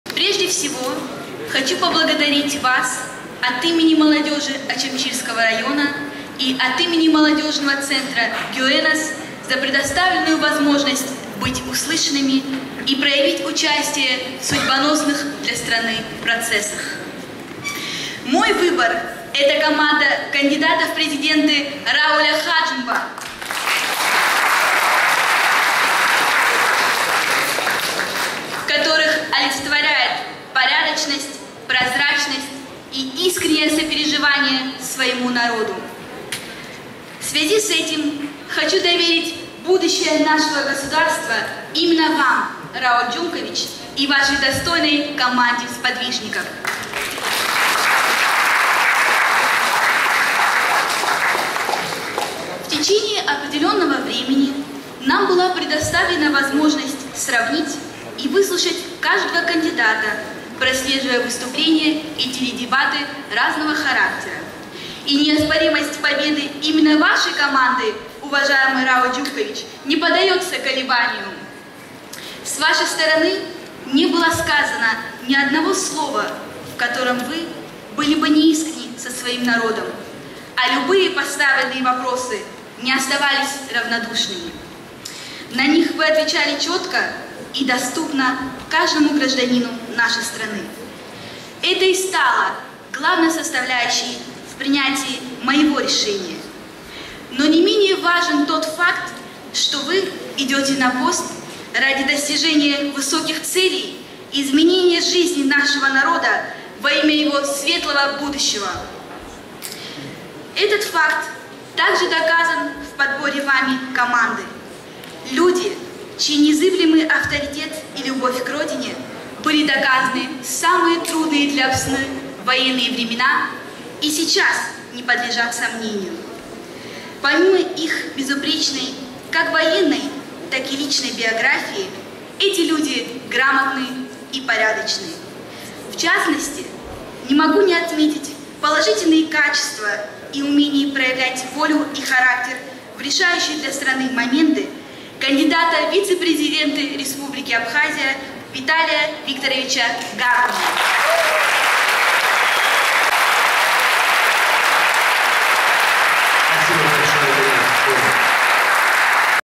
Аминат Ачба
Молодежный форум - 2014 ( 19 августа 2014,г.Сухум,Абхазия)